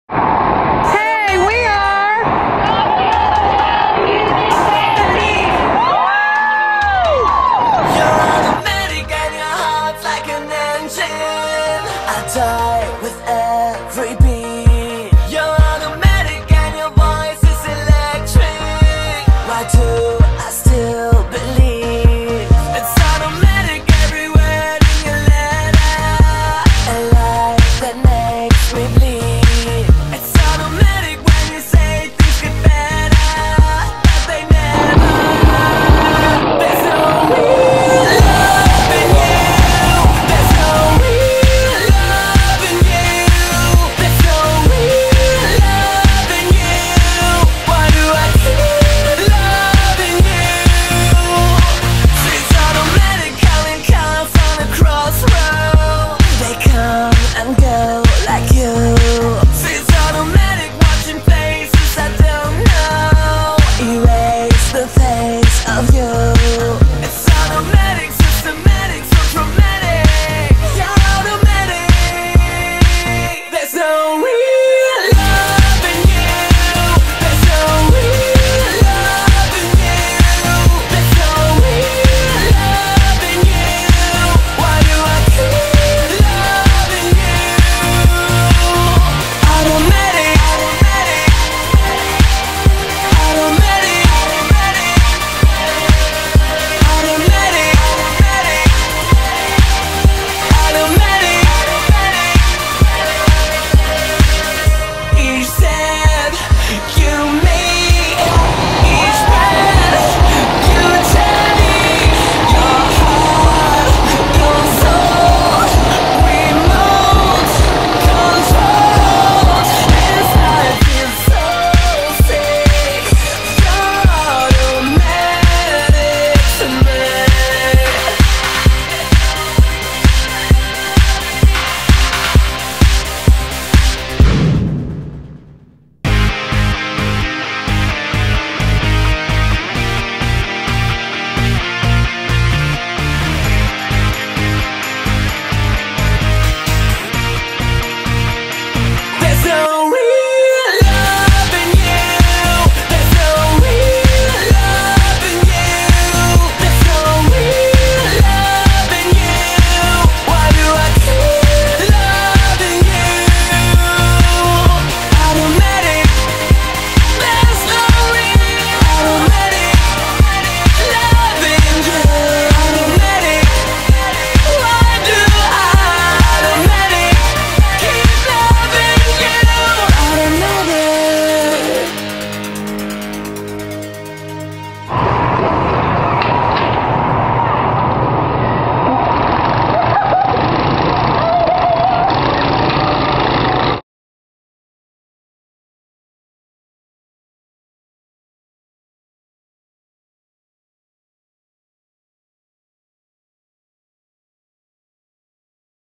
Go kart racing with Tokio Hotel Houston [5-8-2010]
Go kart racing with Tokio Hotel Houston fan team!! We are Tokio Hotel Houston.